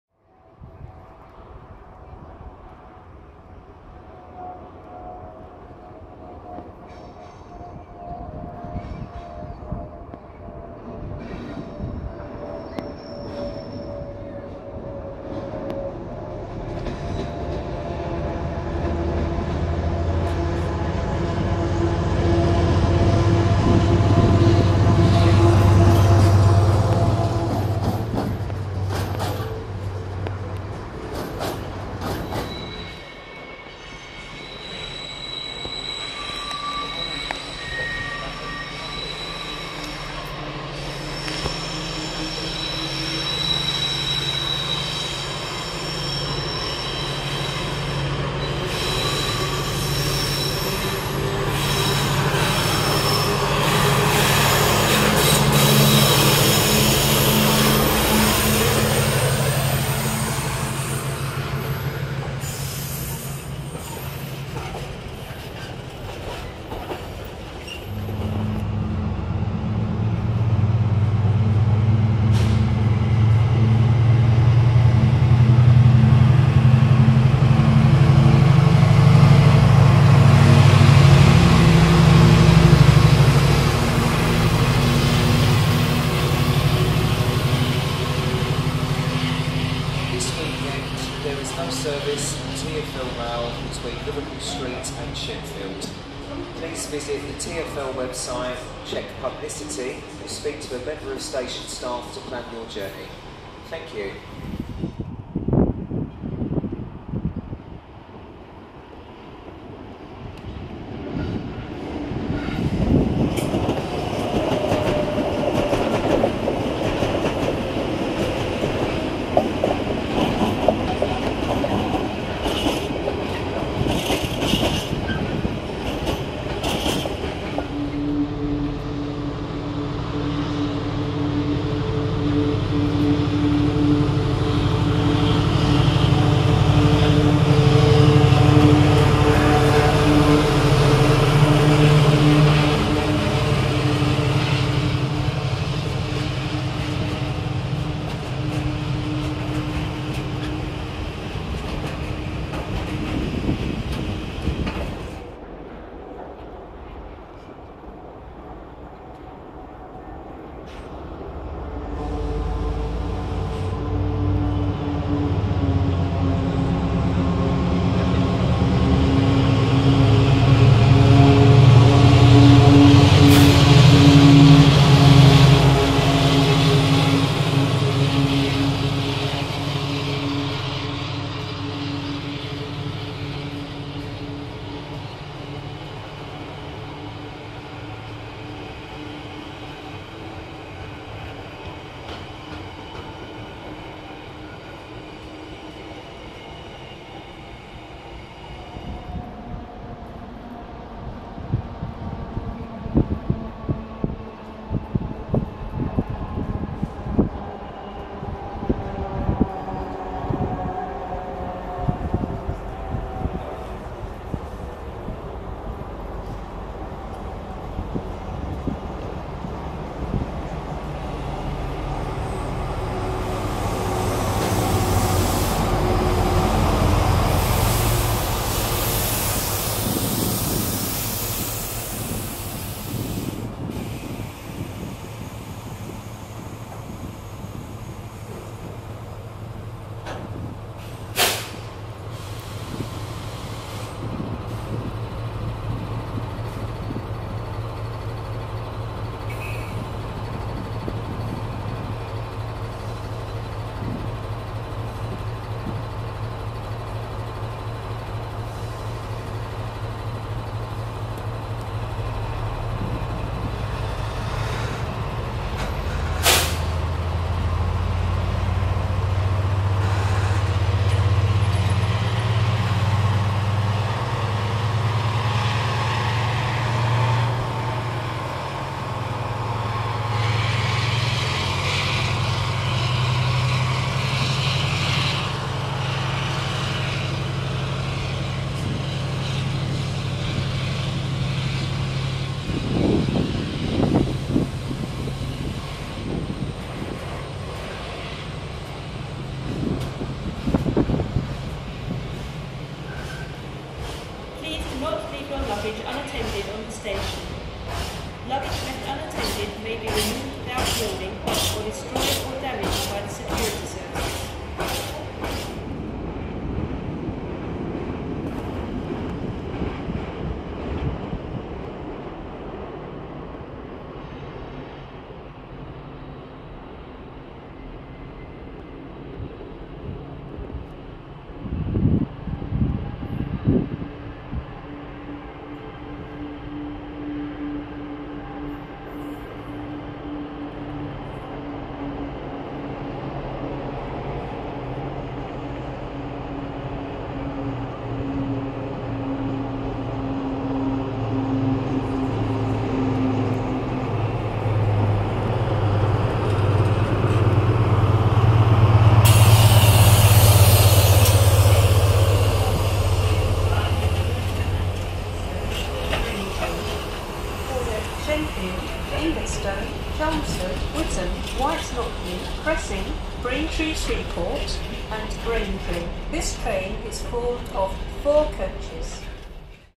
Class 70s at Stratford in 2018
A selection of Freightliner Class 70 - GE Powerhaul - locomotives seen passing Stratford, East London, during 2018. I started recording these locomotives when there was a rumour Freightliner were going to store the few in service.